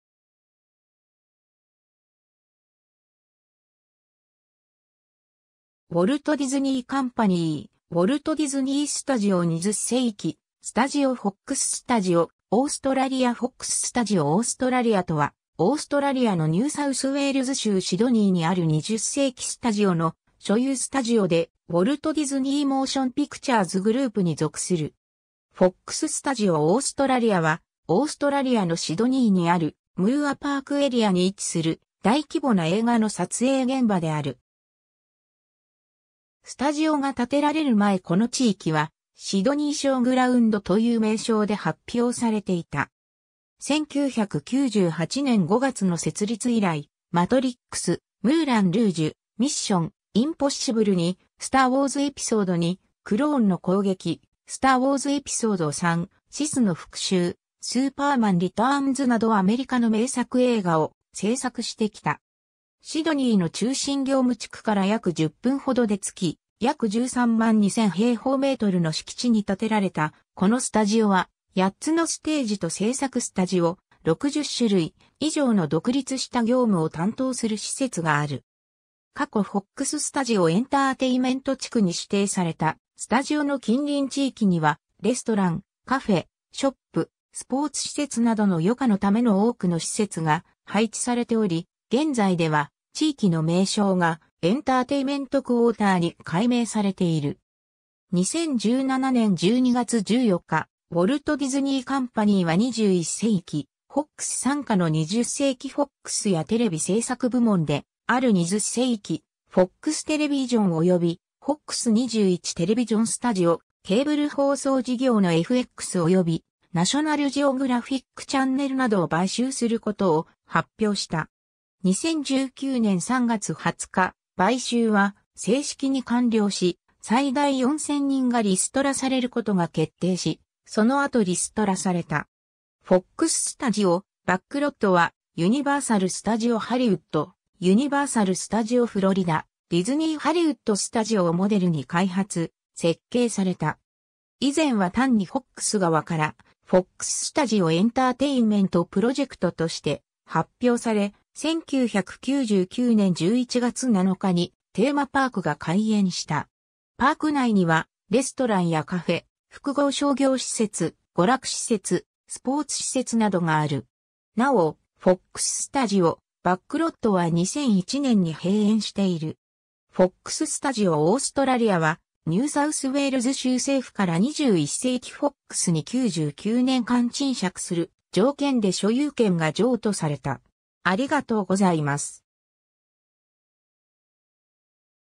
フォックススタジオ・オーストラリア
ウォルト・ディズニー・カンパニー 」 ウォルト・ディズニー・スタジオ 」 20世紀スタジオ 」 フォックススタジオ・オーストラリア フォックススタジオ・オーストラリア（英: Fox Studios Australia）とは、オーストラリアのニューサウスウェールズ州シドニーにある20世紀スタジオ（ウォルト・ディズニー・カンパニー）の所有スタジオで、ウォルト・ディズニー・モーション・ピクチャーズ・グループに属する。
 フォックススタジオ・オーストラリアは、オーストラリアのシドニーにあるムーアパークエリアに位置する、大規模な映画の撮影現場である。
スタジオが建てられる前この地域は、「シドニーショーグラウンド」という名称で発表されていた。
 1998年5月の設立以来『マトリックス』、『ムーラン・ルージュ』、『ミッション:インポッシブル2』、『スター・ウォーズ エピソード2/クローンの攻撃』、『スター・ウォーズ エピソード3/シスの復讐』、『スーパーマン リターンズ』などアメリカの名作映画を製作してきた。
 シドニーの中心業務地区から約10分ほどで着き、約 132,000 m2 の敷地に建てられたこのスタジオは、8つのステージと制作スタジオ、60種類以上の独立した業務を担当する施設がある。
 過去フォックススタジオ・エンターテイメント地区に指定されたスタジオの近隣地域には、レストラン、カフェ、ショップ、スポーツ施設などの余暇のための多くの施設が配置されており、現在では地域の名称がエンターテイメントクォーターに改名されている。
 2017年12月14日、ウォルト・ディズニー・カンパニーは21世紀フォックス傘下の20世紀フォックスやテレビ製作部門である20世紀フォックステレビジョンおよびフォックス21 テレビジョン・スタジオ、ケーブル放送事業のFXおよびナショナルジオグラフィックチャンネルなどを買収することを発表した。
2019年3月20日、買収は正式に完了し、最大4000人がリストラされることが決定し、その後リストラされた。
 フォックススタジオ・バックロットは、ユニバーサル・スタジオ・ハリウッド、ユニバーサル・スタジオ・フロリダ（ユニバーサル・オーランド・リゾート）、ディズニー・ハリウッド・スタジオ（ウォルト・ディズニー・ワールド・リゾート）をモデルに開発、設計された。
 以前は単にフォックス側から「フォックススタジオ・エンターテインメント・プロジェクト」として発表され、1999年11月7日にテーマパークが開園した。
パーク内には、レストランやカフェ、複合商業施設、娯楽施設、スポーツ施設などがある。
なお、フォックススタジオ・バックロットは2001年に閉園している。
 フォックススタジオ・オーストラリアは、ニューサウスウェールズ州政府から21世紀フォックス（ニューズ・コーポレーション）に99年間賃借する条件で所有権が譲渡された。